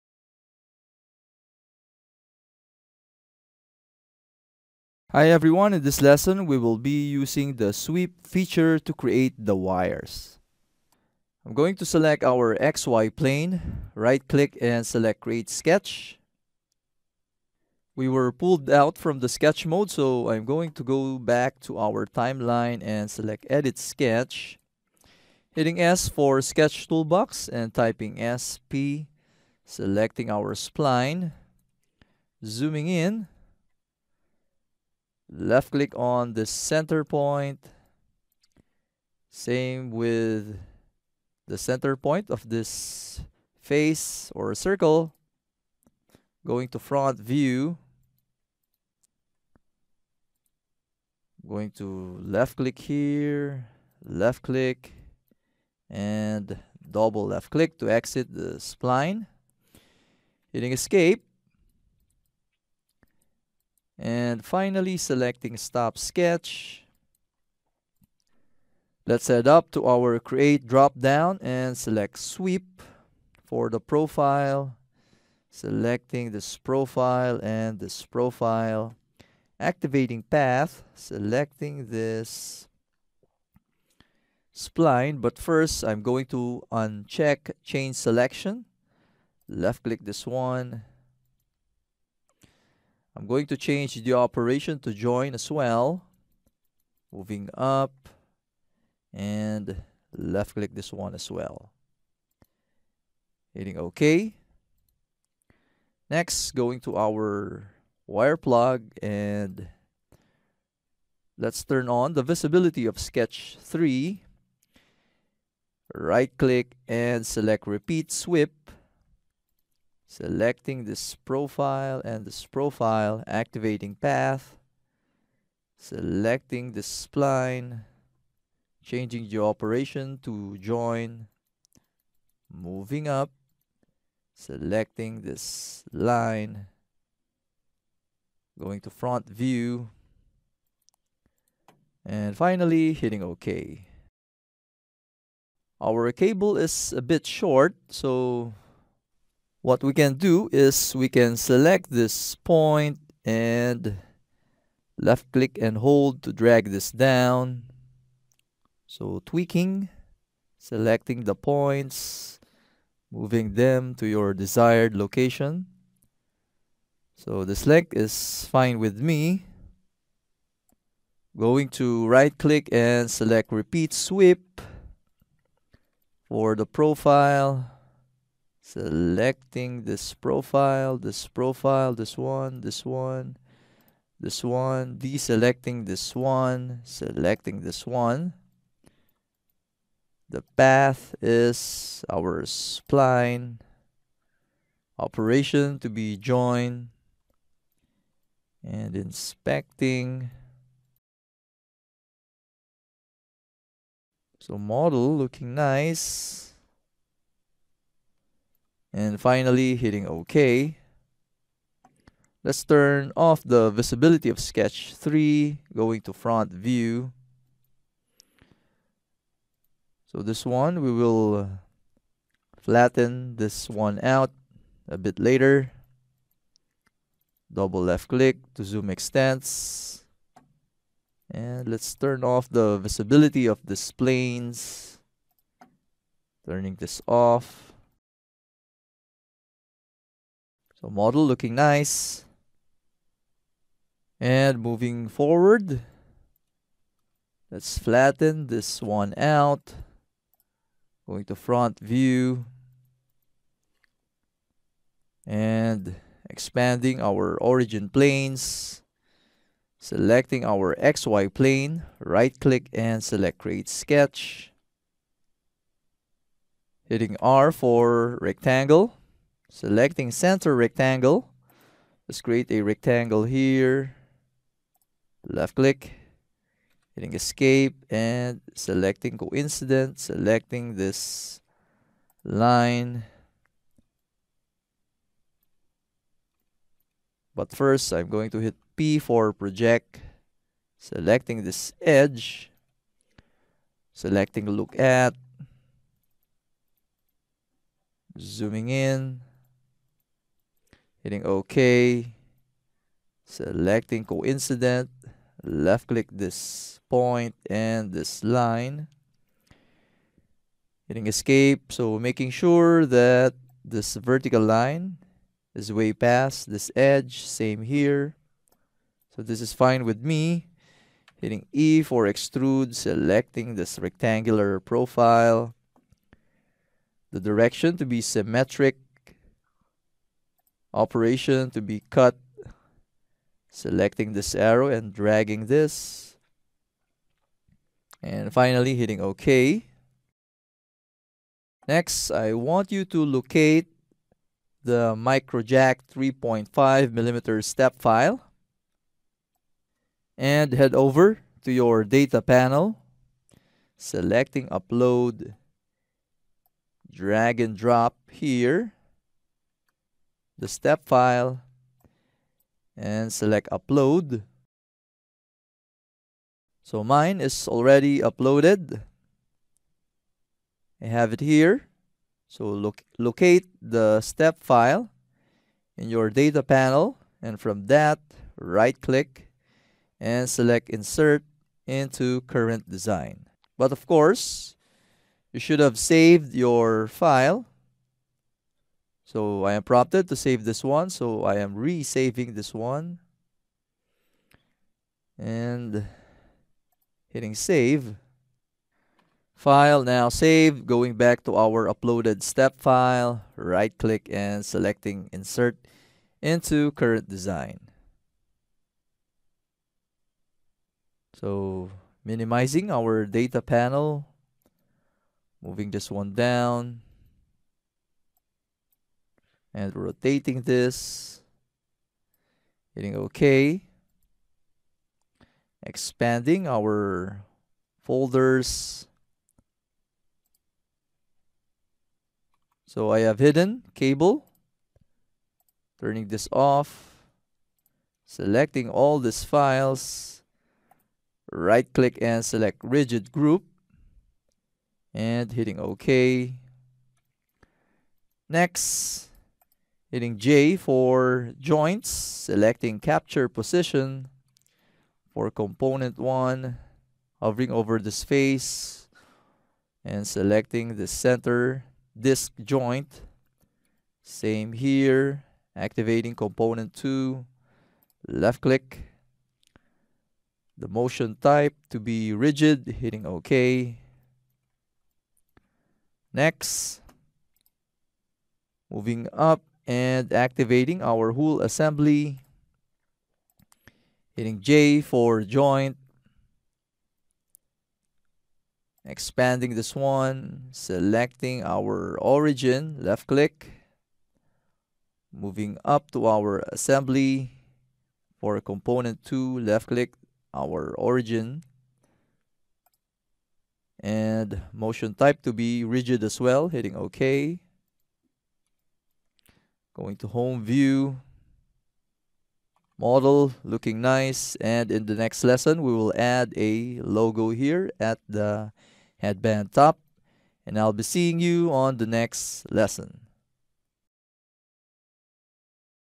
Sweeping the wires
In this excerpt from JMRC'S Udemy course "Fusion 360 Product Concepts: Headphone" course, we will learn how to apply a utilize the sweep command to create the headphone wires in Fusion 360.